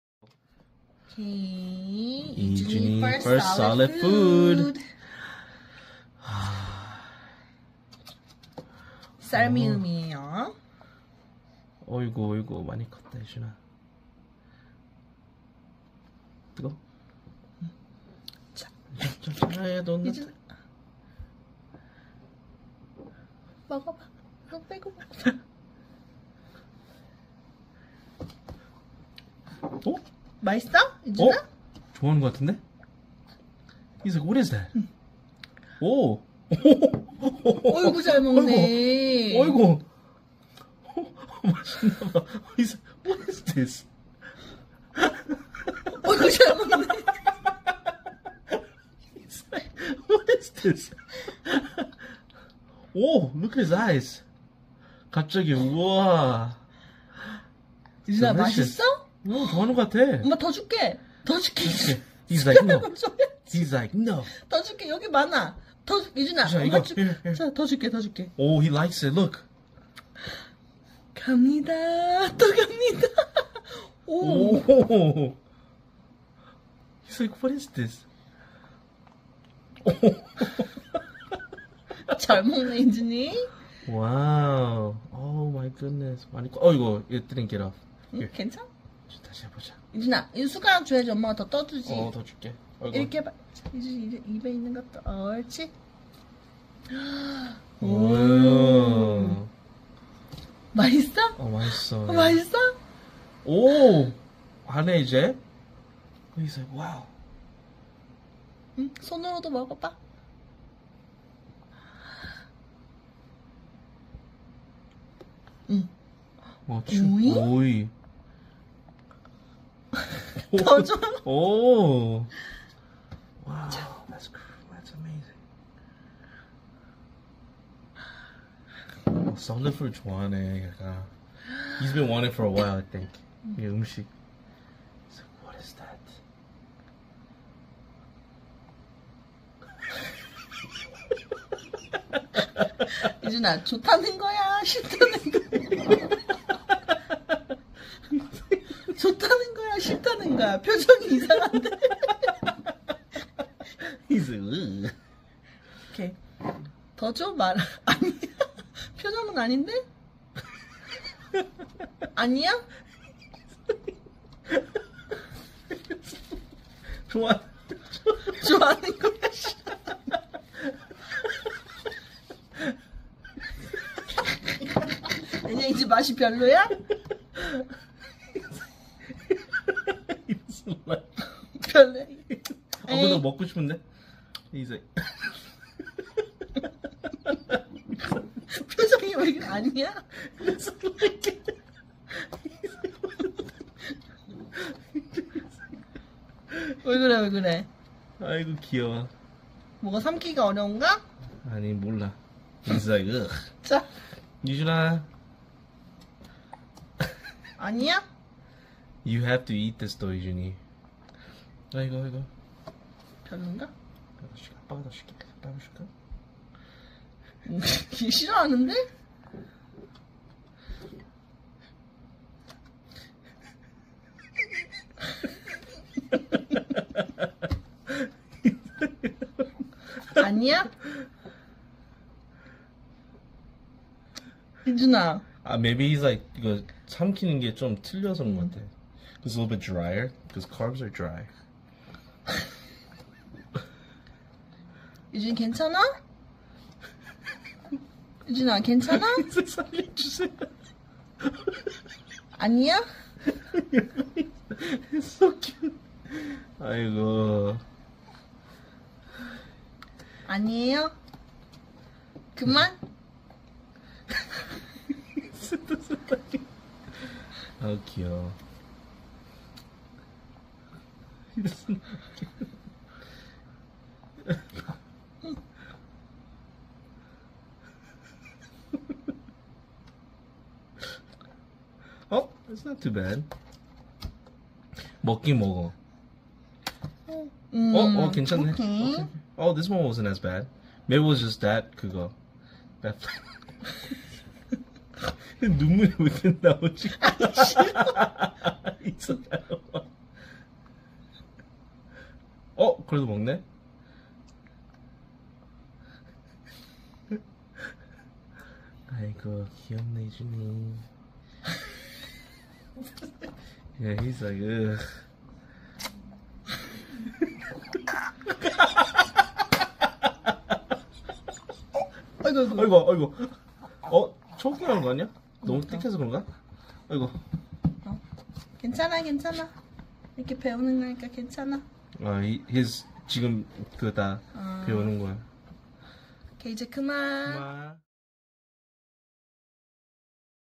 생후 6개월만에 첫 이유식을 맛본 아기의 반응 Yijoon's first solid food
첫 손자 이준이가 생후 6개월만에 처음으로 이유식을 맛보았습니다.